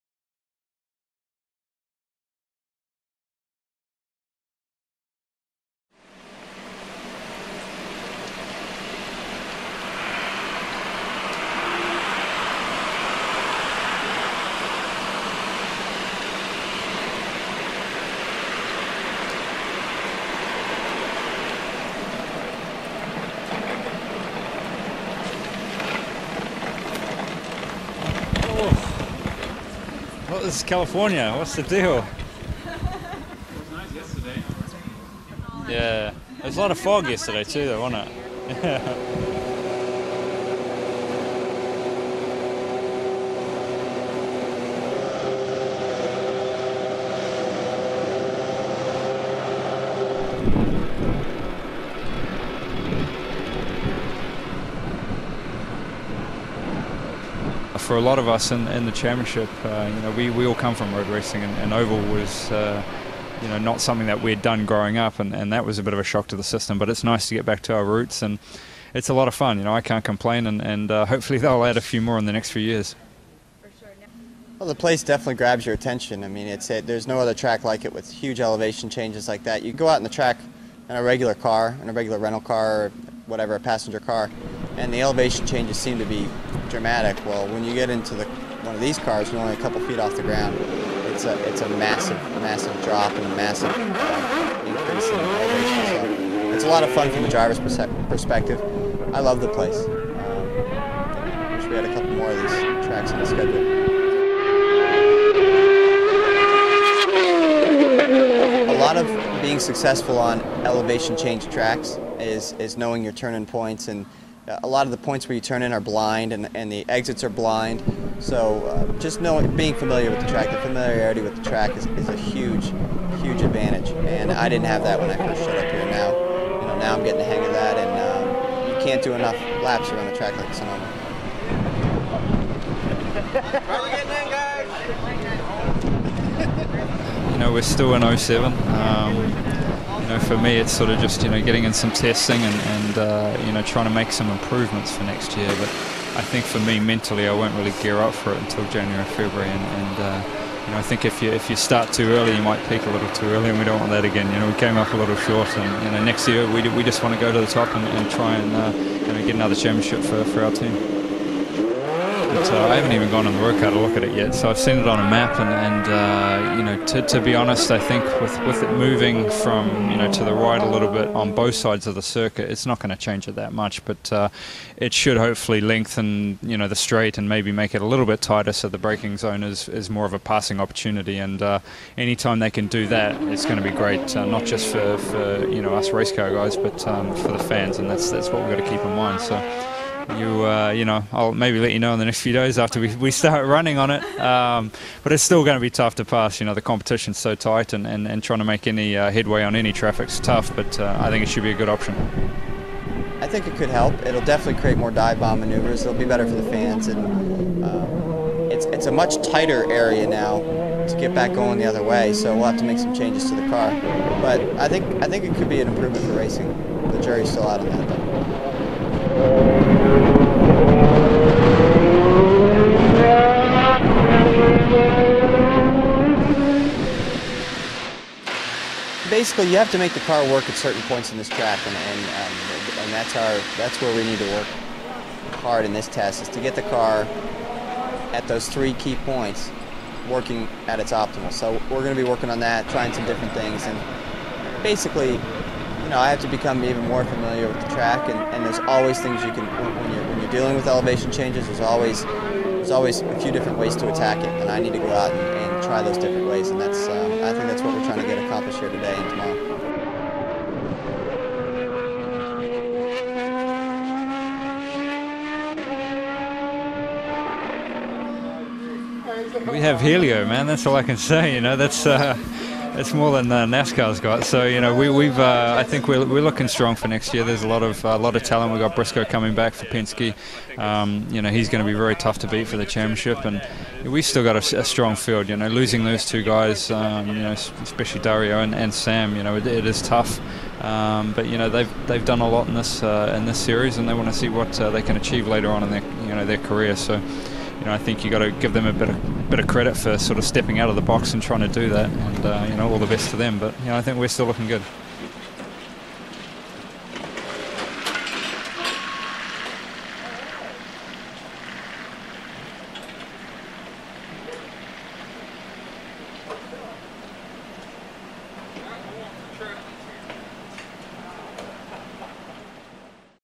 Scott Dixon and Ryan Hunter-Reay Test at Infineon Raceway
IndyCar test session at Infineon Raceway, December 2007. Features interviews with Scott Dixon and Ryan Hunter-Reay.

This is a video that I produced for the official Infineon Raceway webpage